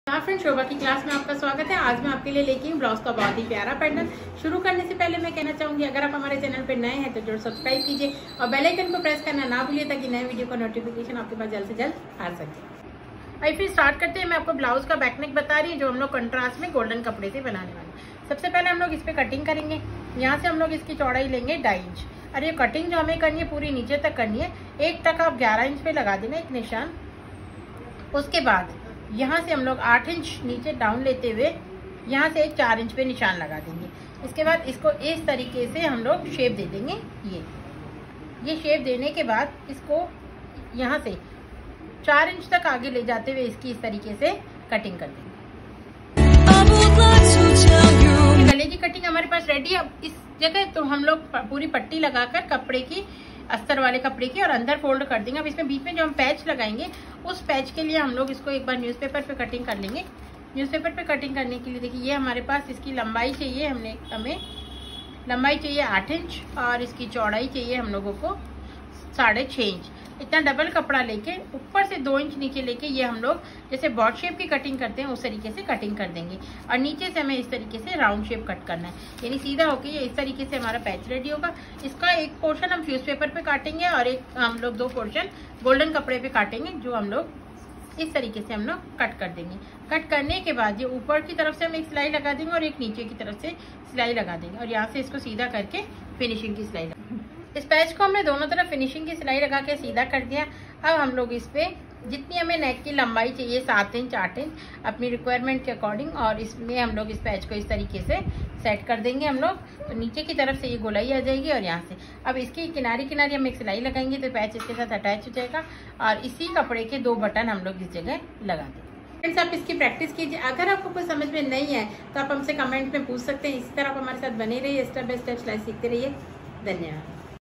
Blouse Design|| Blouse Design 2022
Blouse Design|| Blouse Design 2022|| shobhakiclass blousedesign join our sewing course and learn How to cutting and stitching of double astar(lining) blouse back neck design.

Bottom wear

designer blouse

Drafting

Neckline

sleeves